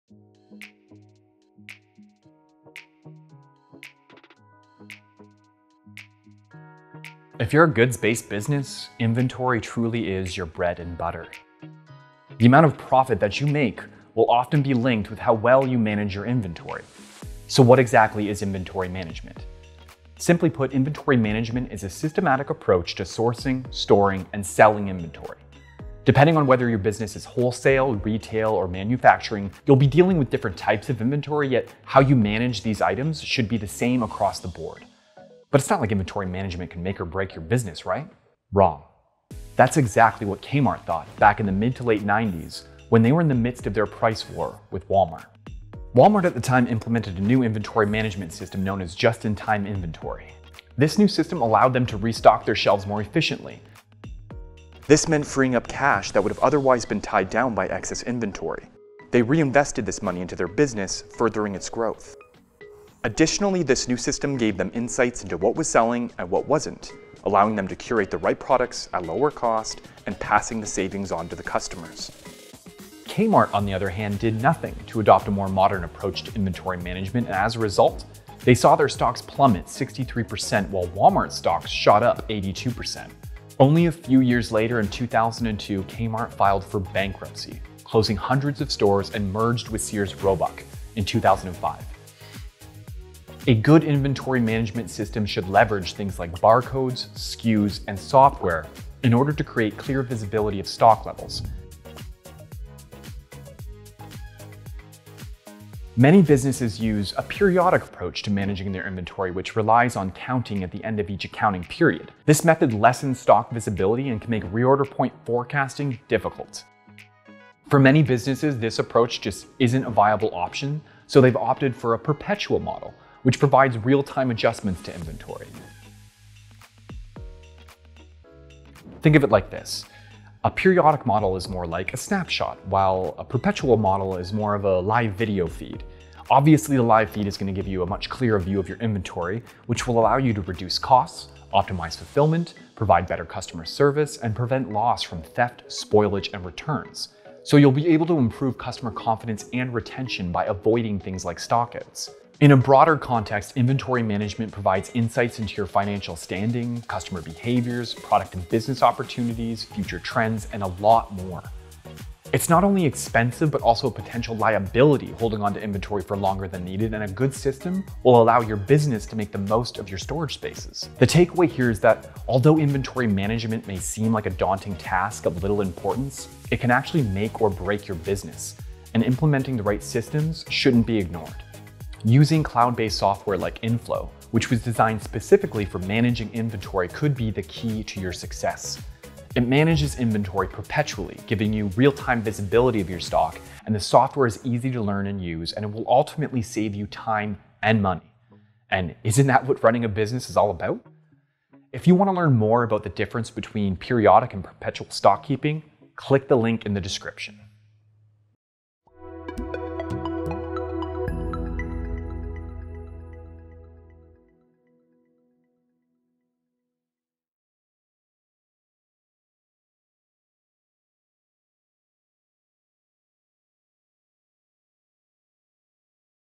What is Inventory Management?
Inventory is about more than just products on a shelf. Proper inventory management is a key aspect for any business and implementing the right system can make all the difference.

0:00 - Warehouse Manufacturing and Inventory Management
0:39 - Just in Time Inventory Anecdote
1:33 -  Aspects of Inventory Management
2:32 - Benefits of Good Inventory Management
3:02 - Benefits of Inventory Software

// Transcript //
If you're a goods based business, inventory truly is your bread and butter. The amount of profit that you make will often be linked with how well you manage your inventory. So what exactly is inventory management? Simply put, inventory management is a systematic approach to sourcing, storing and selling inventory, depending on whether your business is wholesale, retail or manufacturing. You'll be dealing with different types of inventory, yet how you manage these items should be the same across the board. But it's not like inventory management can make or break your business right? Wrong. That's exactly what K-Mart thought back in the mid to late 90s, when they were in the midst of their price war with Wal-Mart. Wal-Mart at the time implemented a new inventory management system known as just in time inventory. This new system allowed them to restock their shelves more efficiently. This meant freeing up cash that would have otherwise been tied down by excess inventory. They reinvested this money into their business, furthering its growth. Additionally, this new system gave them insights into what was selling and what wasn't allowing them to curate the right products at a lower cost and passing the savings on to the customers. Kmart, on the other hand, did nothing to adopt a more modern approach to inventory management. As a result, they saw their stocks plummet sixty three percent, while Wal-Mart stocks shot up 82 percent. Only a few years later, in 2002, Kmart filed for bankruptcy, closing hundreds of stores and merged with Sears Roebuck in 2005. A good inventorymanagementsystem should leverage things like barcodes, SKUs and software in order to create clear visibility of stock levels. Many businesses use a periodic approach to managing their inventory, which relies on counting at the end of each accounting period. This method lessens stock visibility and can make reorder point forecasting difficult for many businesses, this approach just isn't a viable option. So they've opted for a perpetual model, which provides real time adjustment to inventory. Think of it like this, a periodic model is more like a snapshot, while a perpetual model is more of a live video feed. Obviously, the live feed is going to give you a much clearer view of your inventory, which will allow you to reduce costs, optimize fulfillment, provide better customer service and prevent loss from theft spoilage and returns. So you'll be able to improve customer confidence and retention by avoiding things like stock outs. In a broader context. Inventorymanagement provides insights into your financial standing, customer behaviors, product and business opportunities, future trends and a lot more. It's not only expensive, but also a potential liability. Holding on to inventory for longer than needed and a good system will allow your business to make the most of your storage spaces. The takeaway here is that although inventory management may seem like a daunting task of little importance, it can actually make or break your business, and implementing the right systems shouldn't be ignored. Using cloud based software like inFlow, which was designed specifically for managing inventory, could be the key to your success. It manages inventory, perpetually giving you real time visibility of your stock, and the software is easy to learn and use, and it will ultimately save you time and money and isn't that what running a business is all about.